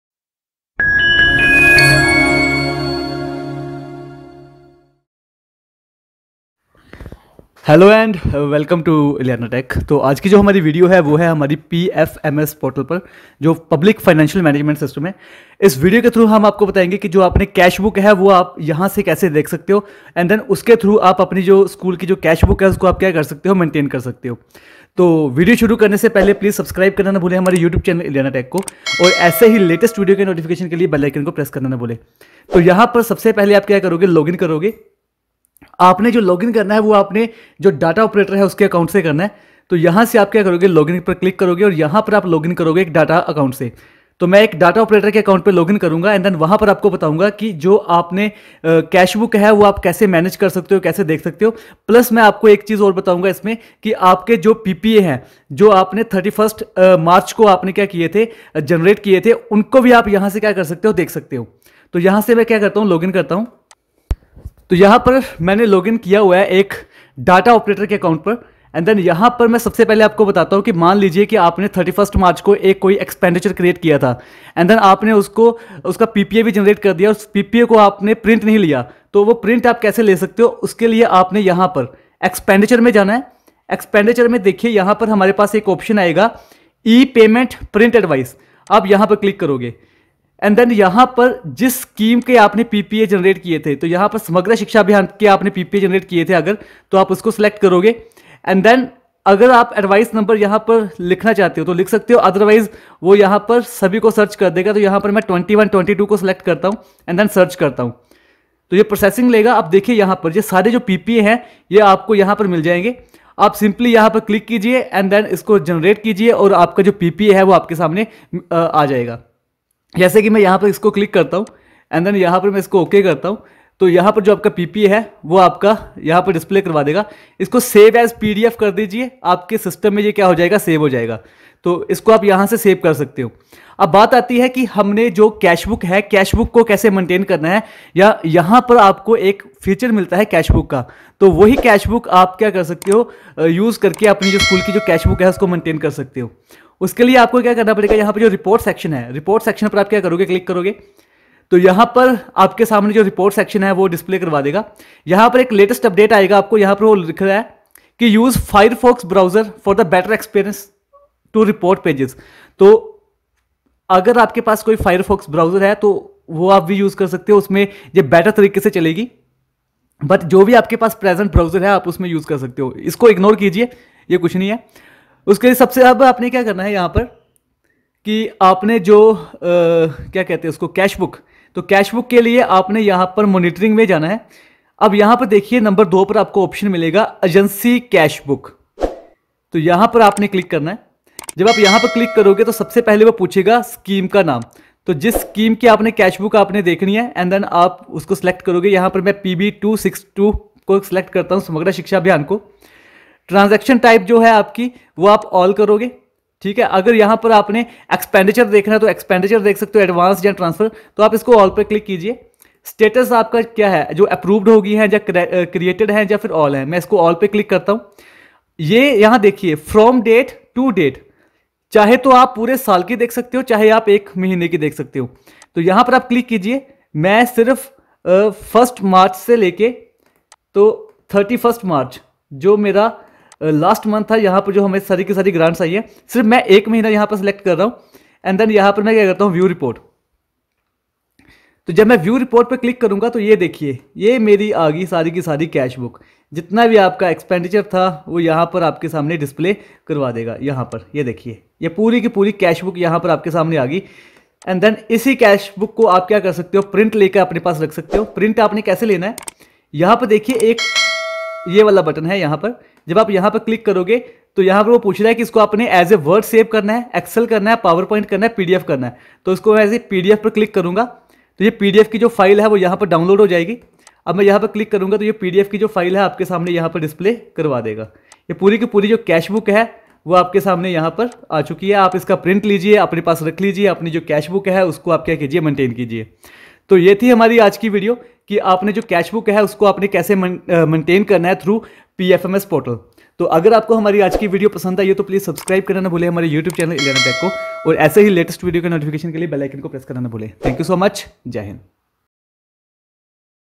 How To Manage Cash Book on PFMS || Reprint PPA || iLeana Tech
Cash Book Maintain कैसे करें
 Cashbook का Print कैसे ले
 How To Reprint PPA

Team iLeana Tech
Some Important Video Link as Following :-